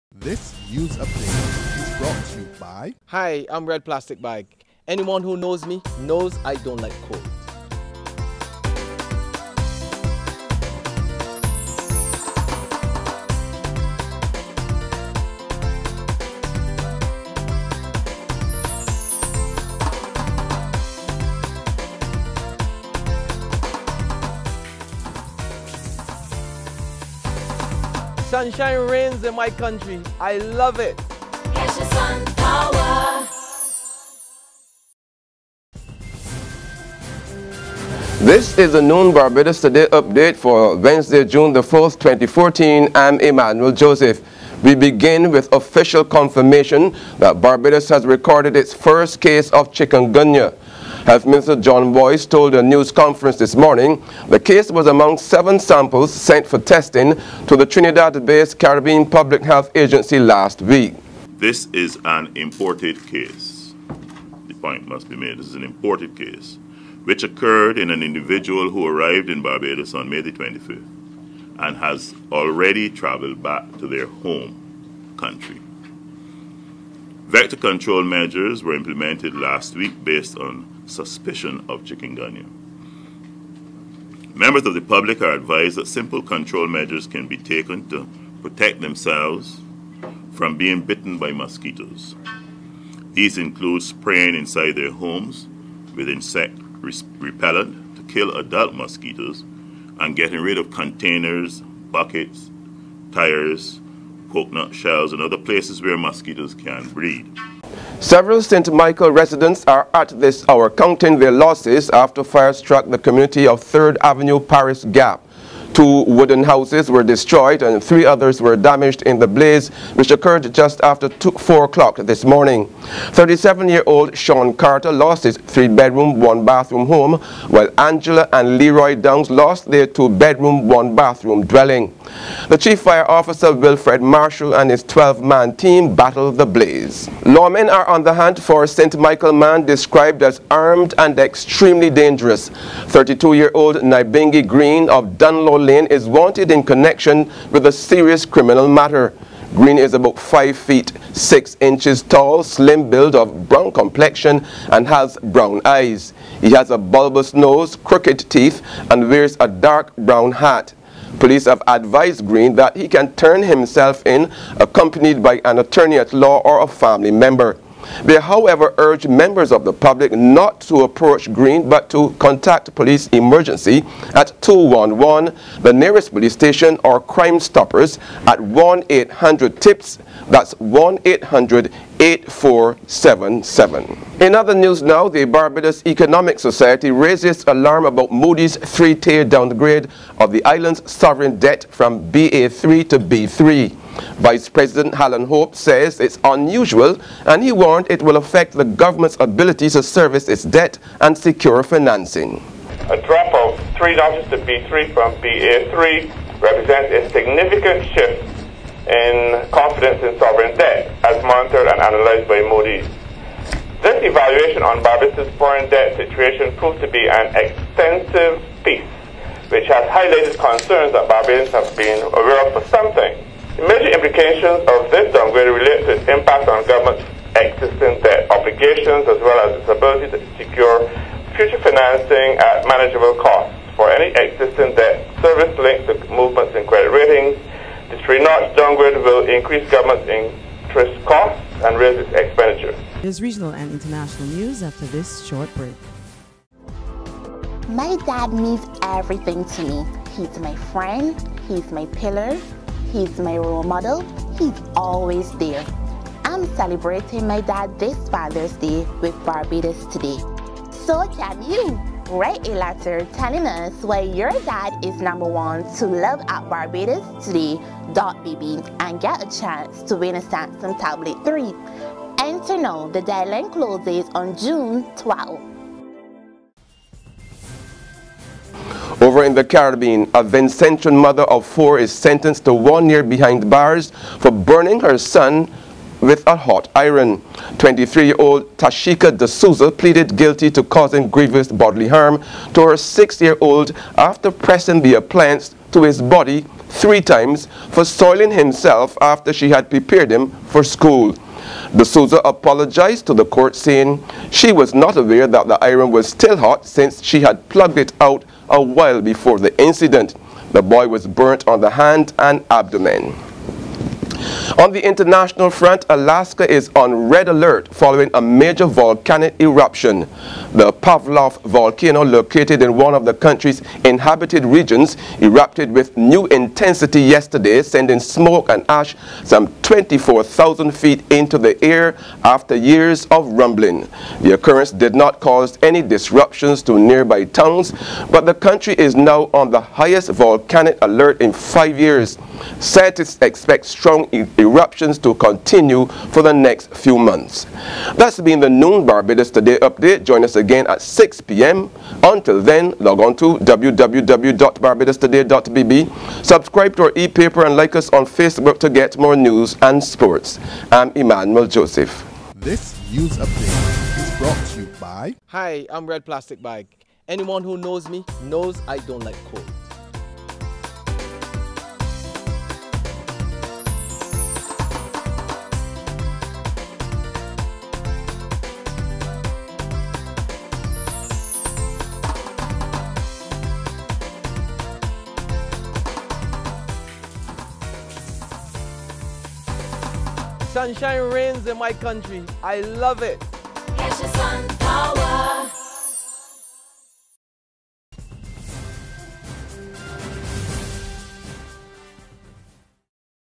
12 PM NEWS UPDATE- JUNE 04, 2014
Chikungunya reaches Barbados.
Fire destroys three houses and damages two others in St. Michael.
Police search for wanted man
...and more in the noon update.